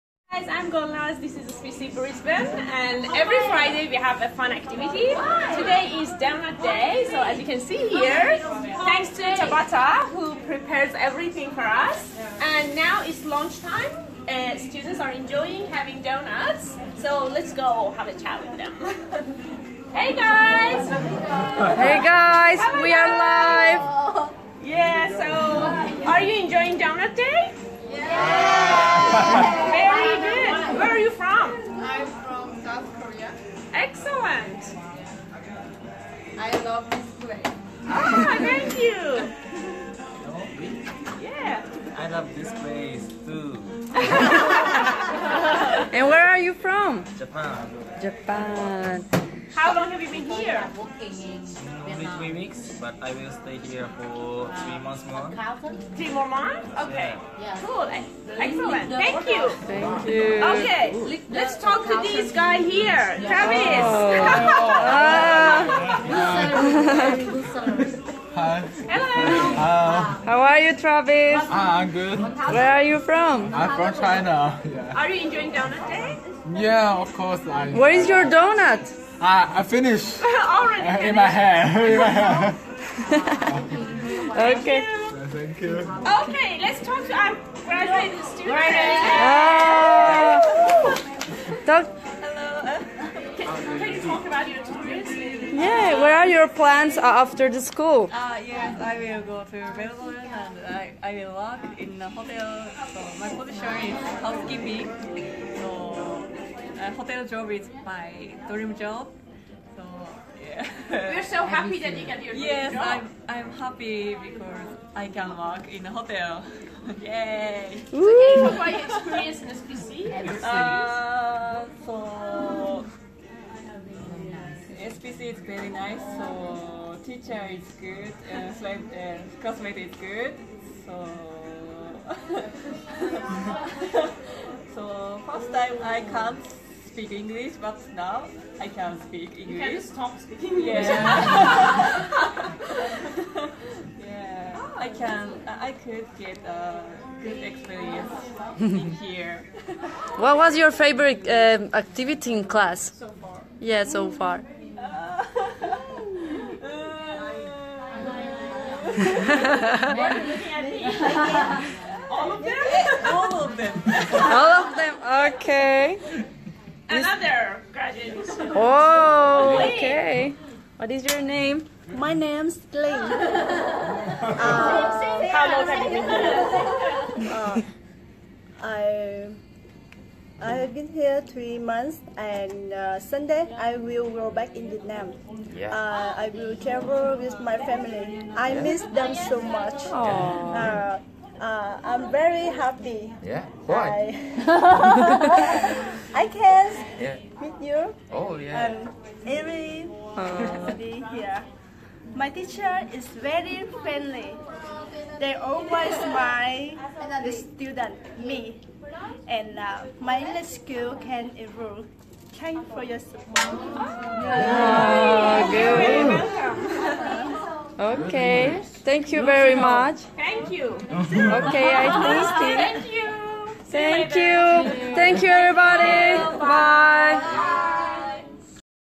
[SPC Brisbane] Doughnut Day LIVE
A great day sharing delicious Doughnuts in SPC Brisbane